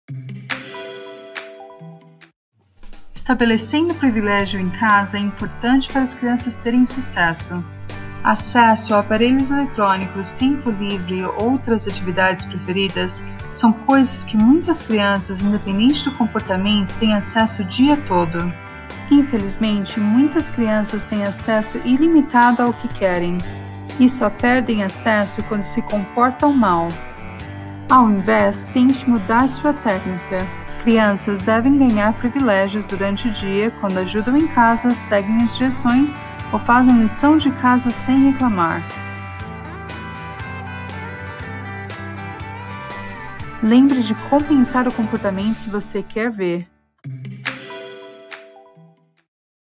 Dica de Comportamento do BRST de hoje é: Estabelecendo privilégios em casa
A dica de comportamento do BRST de hoje é como estabelecer privilégios em casa para aumentar o comportamento que você deseja em casa.